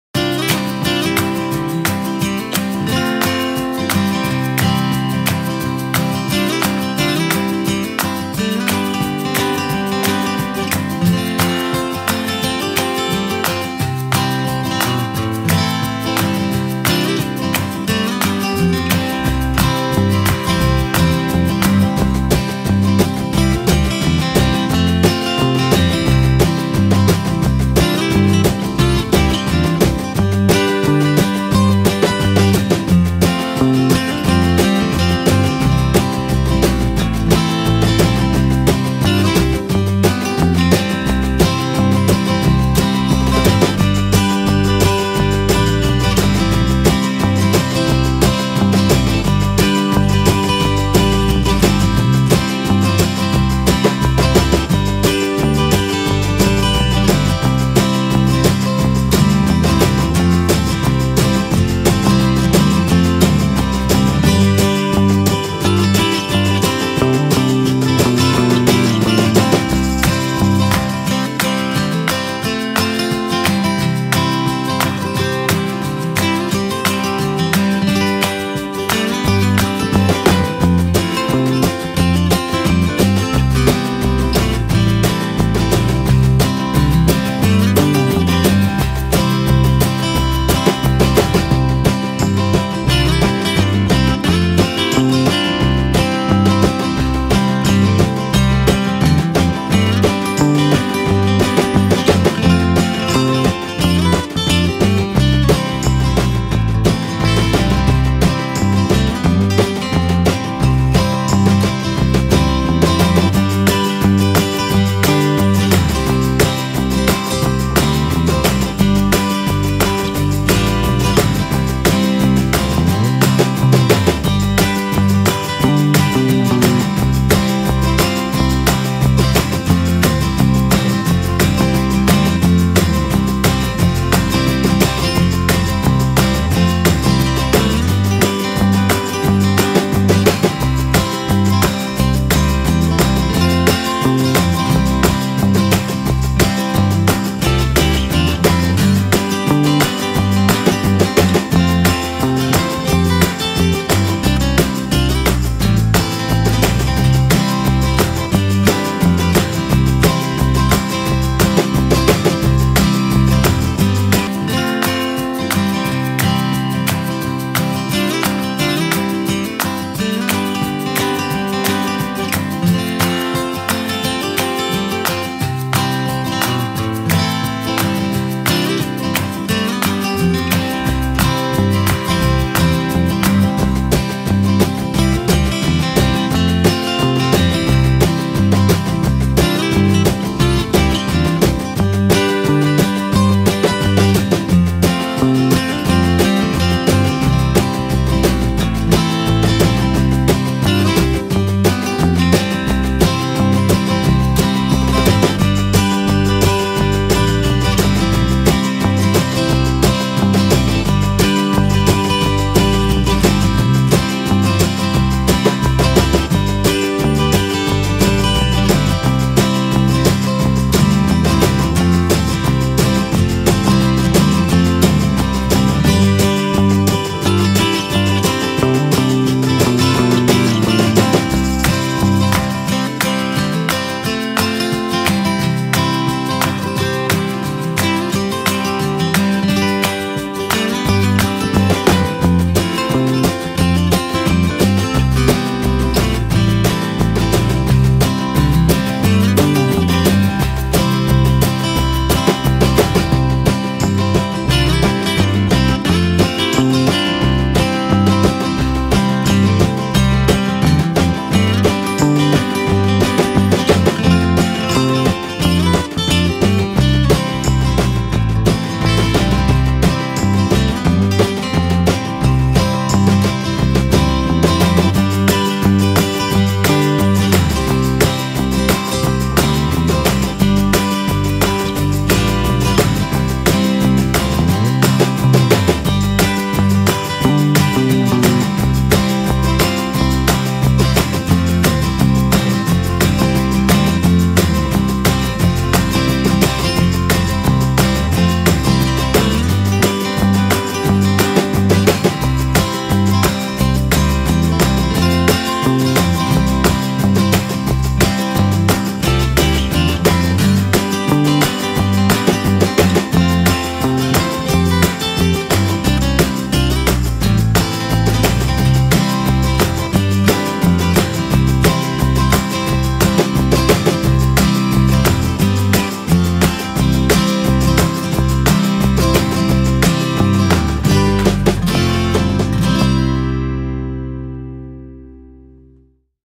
Family Fun at Cultus Lake Waterpark and Adventure Park
We had a blast at Cultus Lake Adventure Park and Cultus Lake Waterpark. The park is celebrating its 35th birthday and it's better than ever.